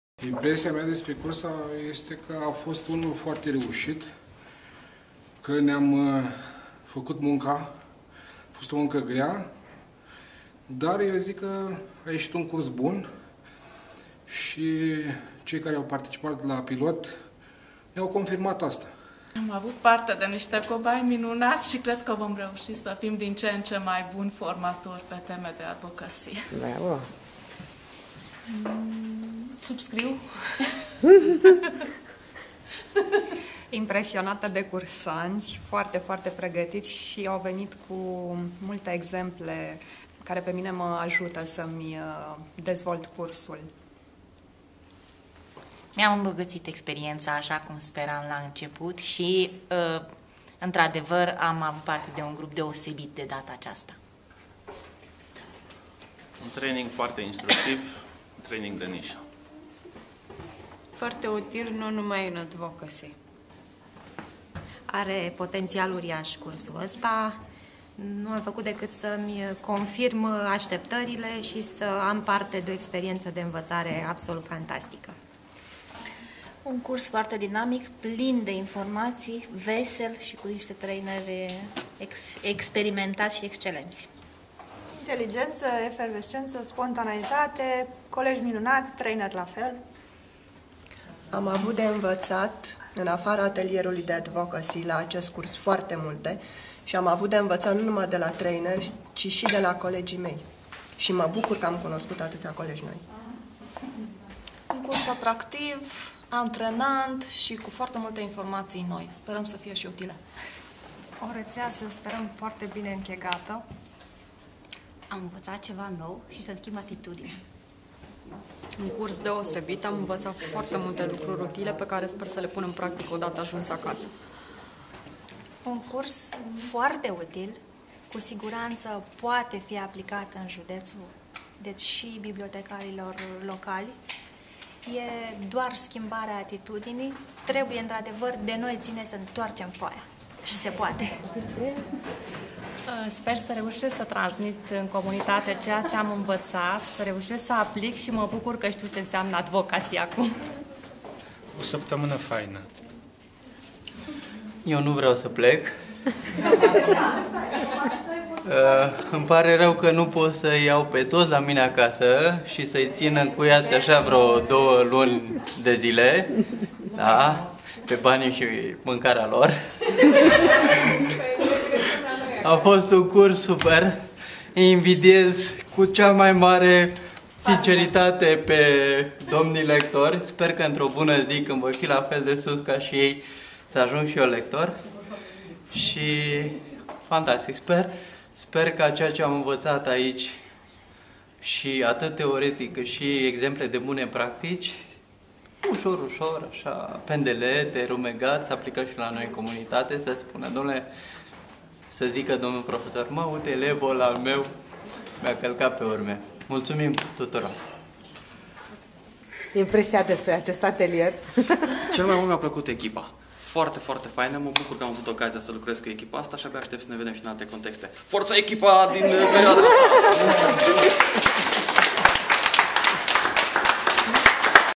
Curs pilot "Advocacy pentru biblioteci puternice si comunitati unite"
Cursul pilot de advocacy pentru biblioteci, desfasurat in saptamana 16-20 iulie, a reunit 14 bibliotecari - reprezentanti ai filialelor ANBPR din zece judete si personalul biroului operational. Cele zece biblioteci judetene care au aplicat pentru a participa sunt: Arges, Braila, Buzau, Constanta, Galati, Gorj, Mures, Prahova, Satu Mare si Vrancea.